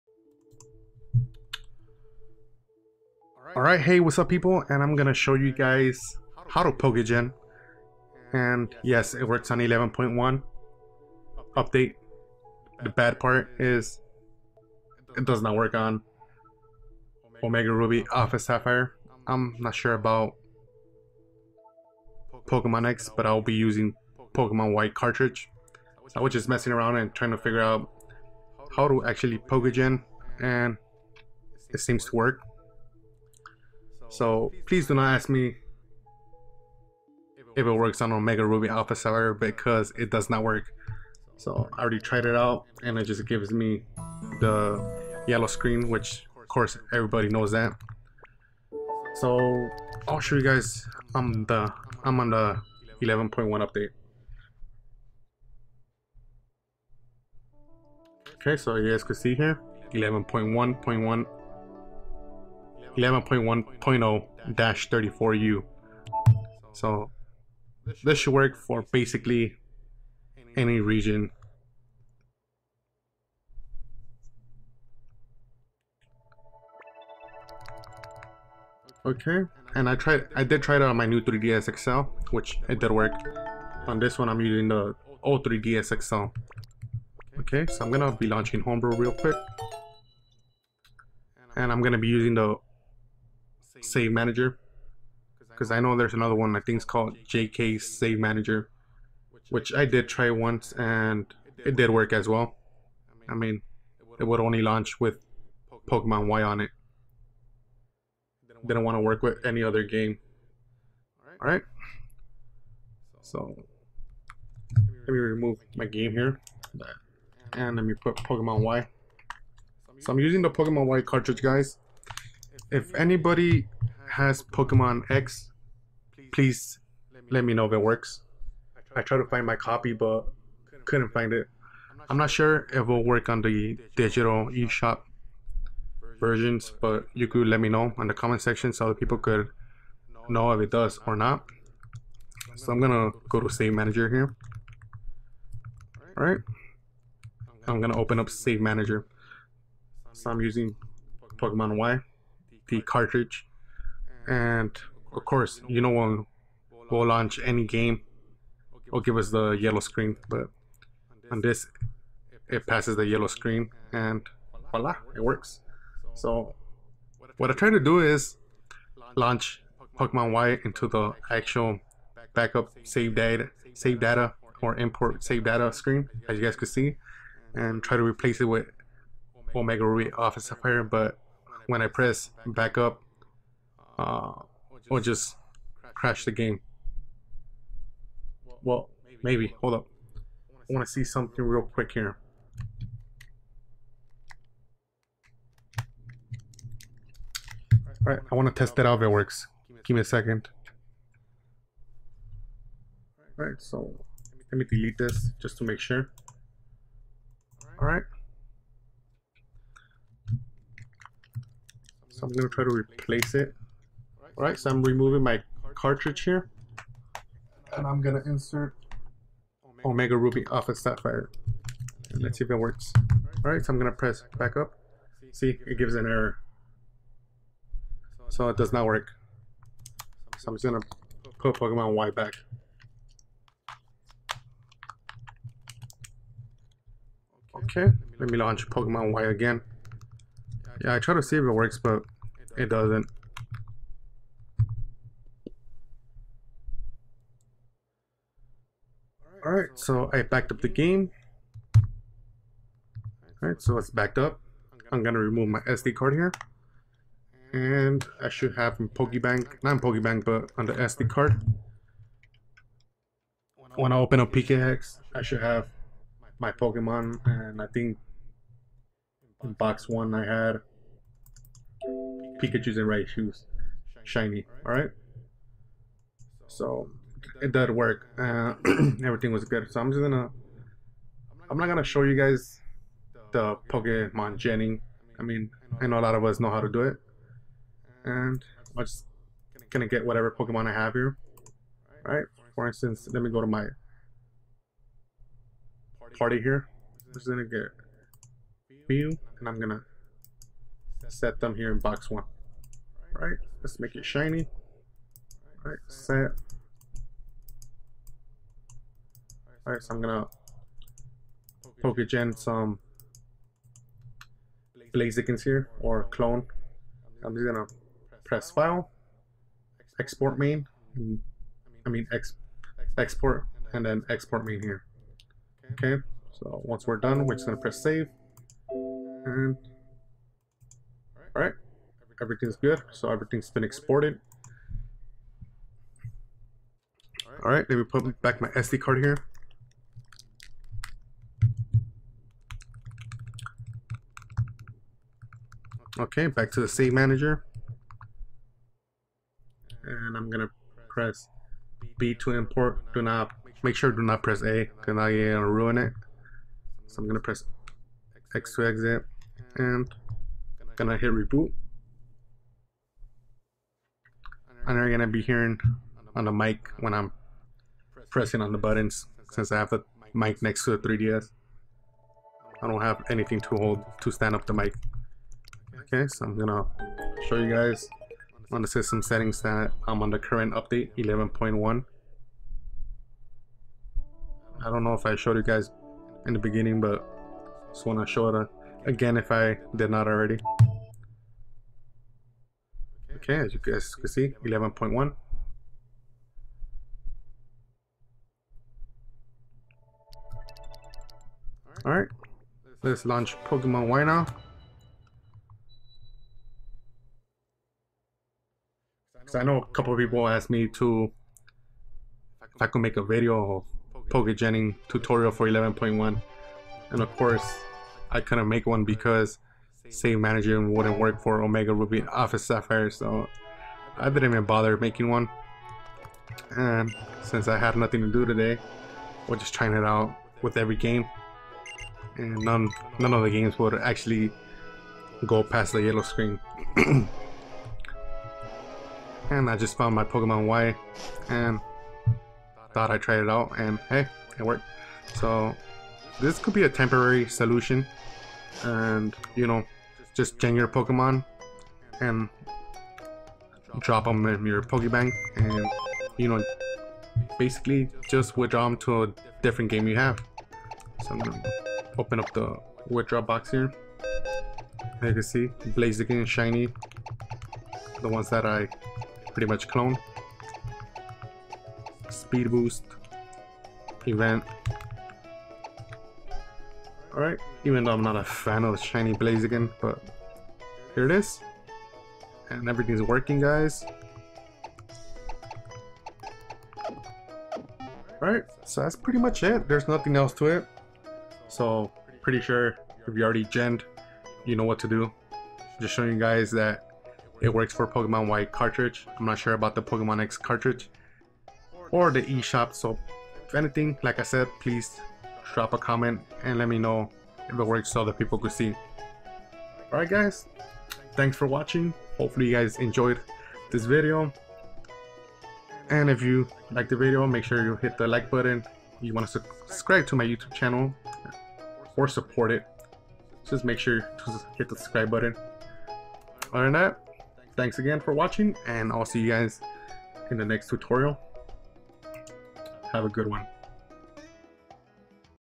How To PokeGen On PKHEX 11.1.0-34 2DS 3DS New 3DS - Temporary Solution
Just figured out a Temporary Solution mean while everything gets updated and functional for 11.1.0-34. I'm using Pokemon Y cartridge, from there you can access pkhex and Poke-Gen any Pokemon then transfer to PokeBank or trade to another Pokemon Game which is better than nothing.